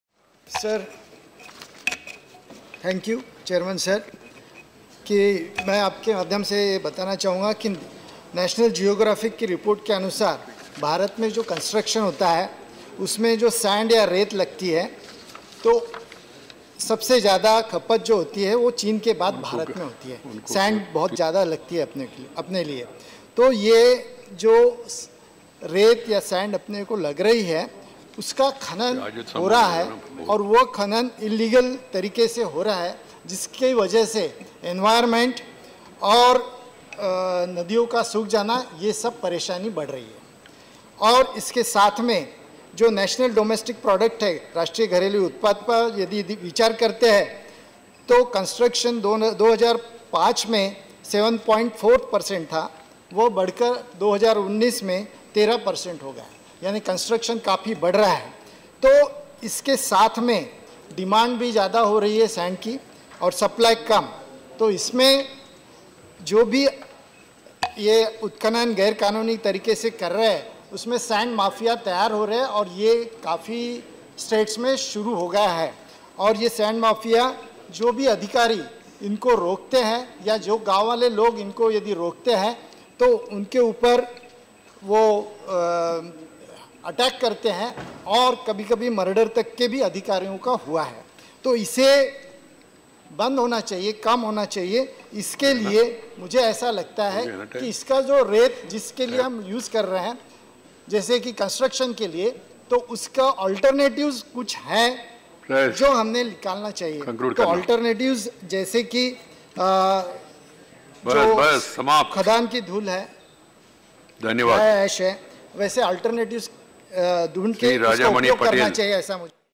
Dr. Vikas Mahatme on Matters Raised With The Permission Of The Chair in Rajya Sabha
Dr. Vikas Mahatme on Matters Raised With The Permission Of The Chair in Rajya Sabha: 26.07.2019